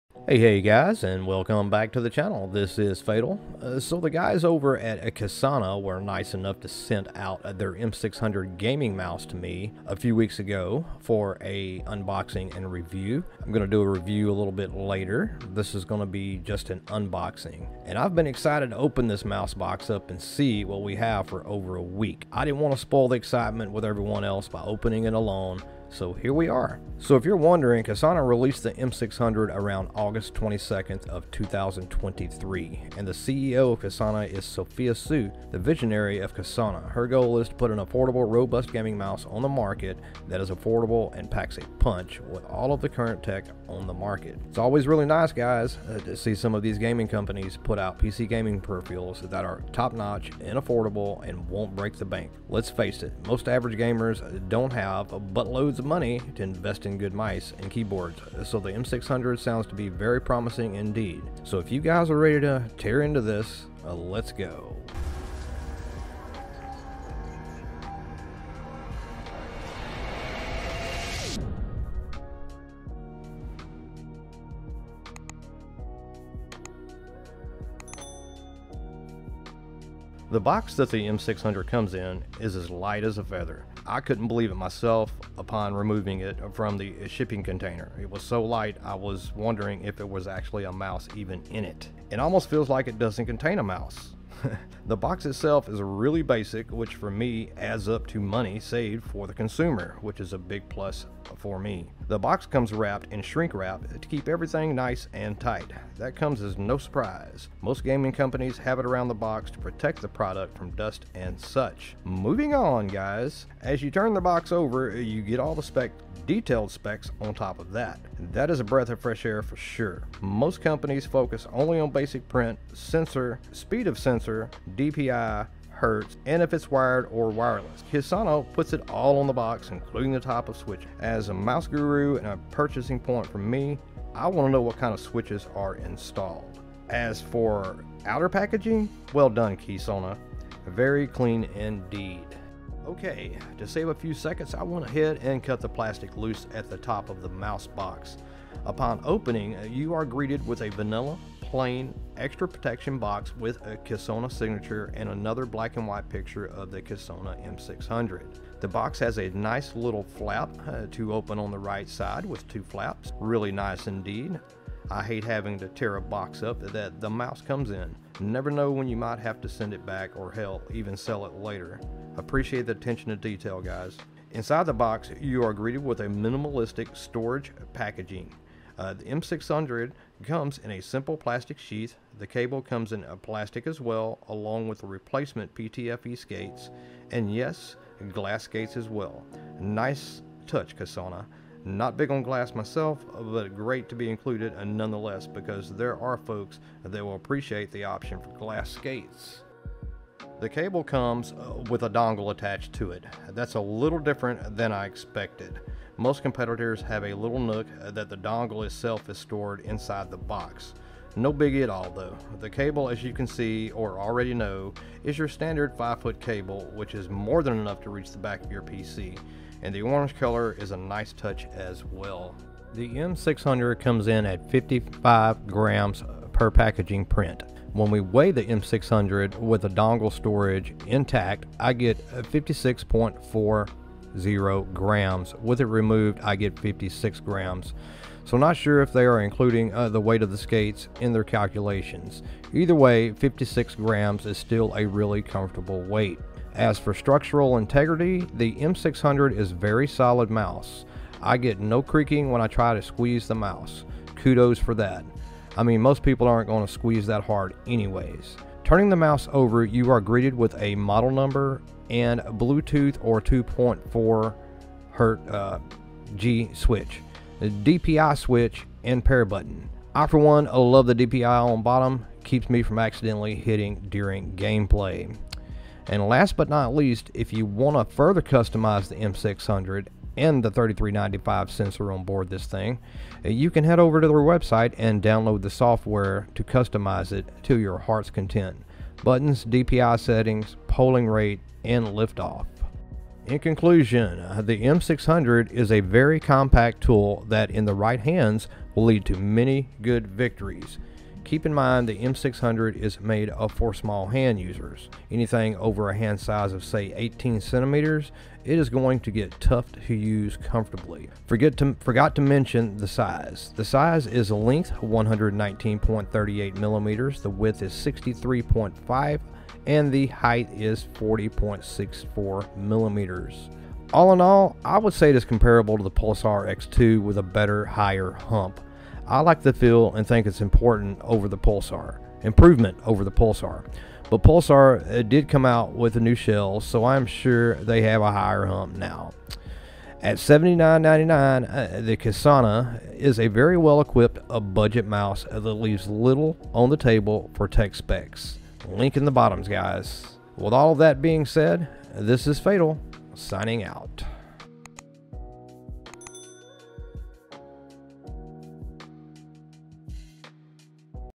TOP WIRELESS MICE 2023 KYSONA M600 WIRELESS GAMING MOUSE UNBOXING AND FIRST IMPRESSIONS
Yoooo. What's up guys. Little ole me again with a new video.

Today we Unbox and go over the @KYSONA M600 Wireless Gaming mouse. The M600 came out back in August and in my opinion is a contender with Pulsar. The bottom of the mouse has a lot of similarities to Pulsar except for one thing, They used  a spot on the bottom of the mouse to store your dongle instead of an empty hole like on the Pulsar mice.

Couple of things I missed putting in the video, Please make sure to remove the the plastic off the skates and the sensor ring. It is there and hard to see.

Also there seems to be a little rise on the sensor area that isn't allowing the mouse to lie flat with the skates. Know that is on a glass pad. On my Cloth pads it's just fine. I am using the Saturn Pro V2 at the moment. Also, The software that you install only allows for dpi increments of 50. And for what ever reason you can not put the dpi at 400, only 450 and also you can't make adjustments to dpi by typing in the number you want. So there is that too.

Other than those few issues the mouse is a solid contender with most top gaming mice.

If you think you are interested in purchasing the M600 here is the link
And also on Amazon

Hope you enjoy the video and be sure to Subscribe if you think I deserve a sub and hit that thumbs up or thumbs down and leave a comment as to your thoughts on the KYSONA M600. Is it a Pulsar Clone? Let me know.